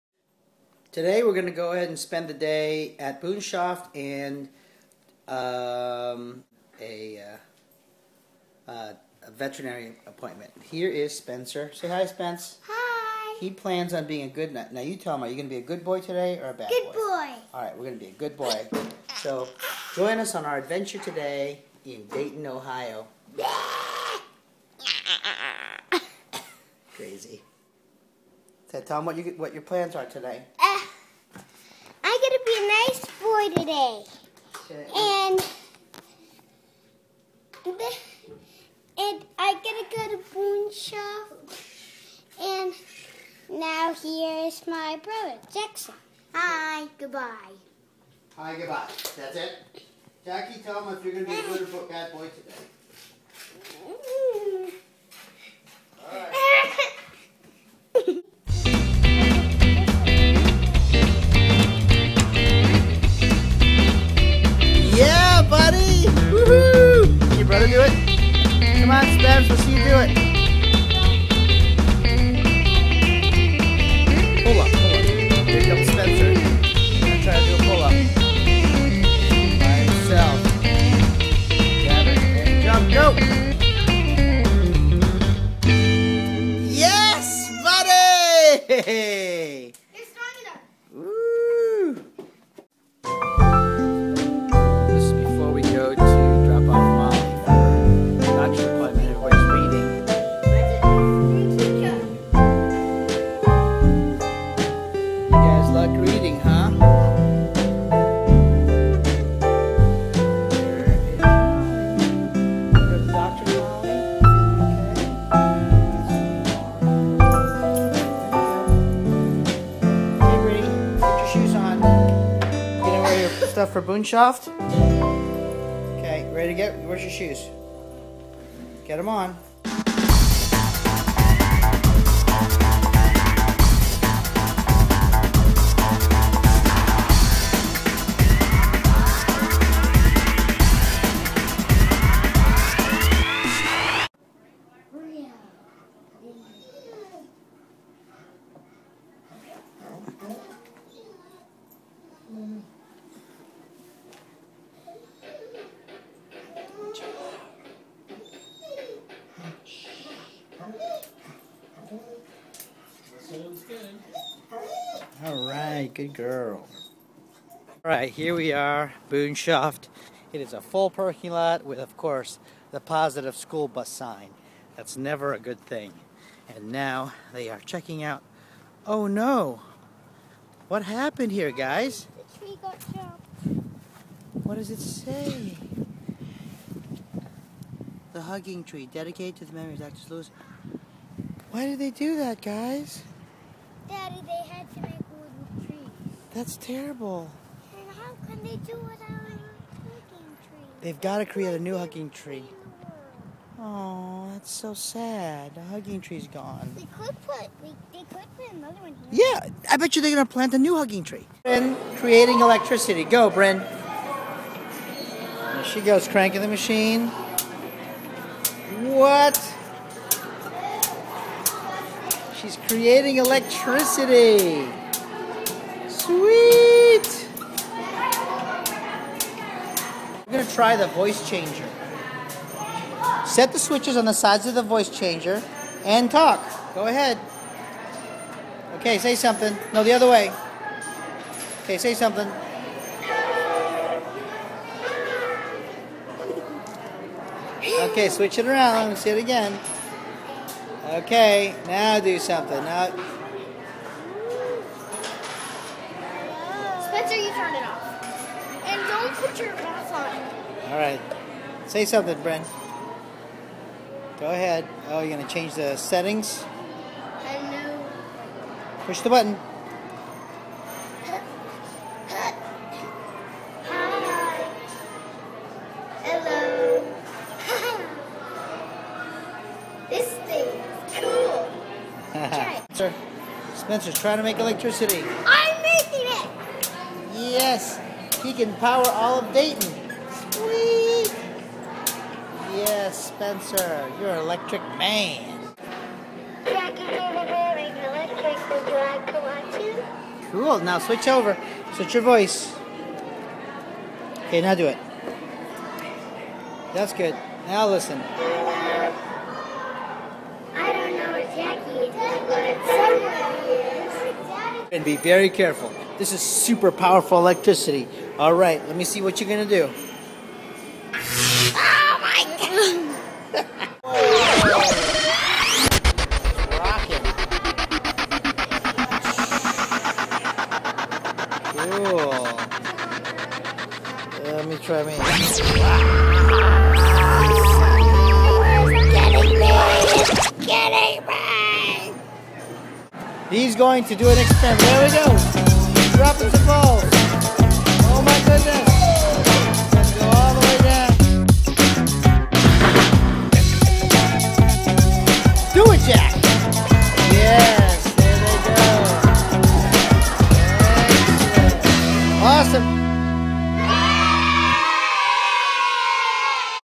Our Day in Dayton Ohio April 2011
Spent the day in Dayton Ohio.  Took Molly to the vet and then went to Boonshoft Children's museum.  April, 2011